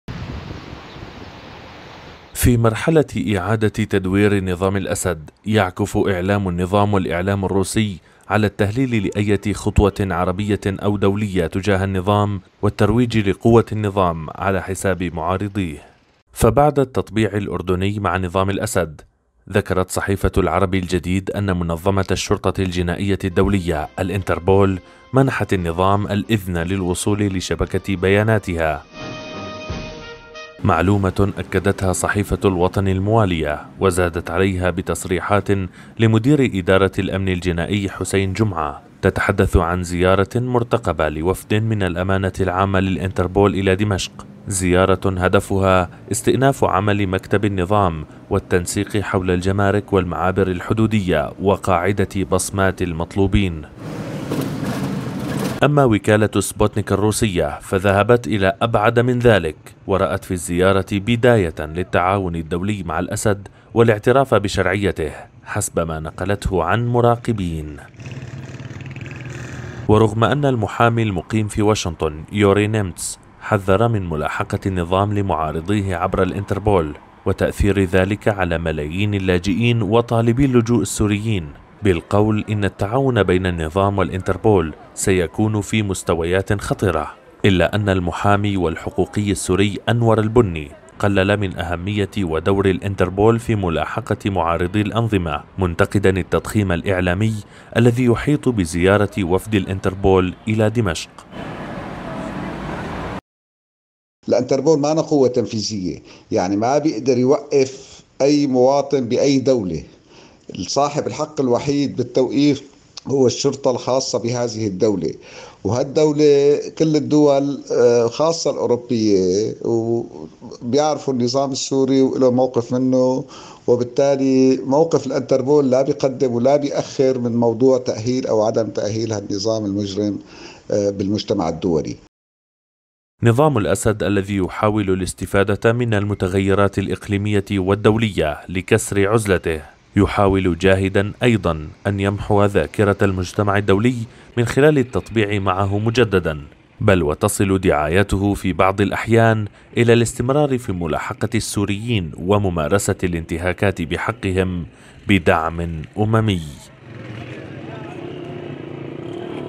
جدل حول ملاحقة الأسد لمعارضيه في الخارج.. مع زيارة مرتقبة لوفد الإنتربول إلى دمشق
تقرير: أمير رضوان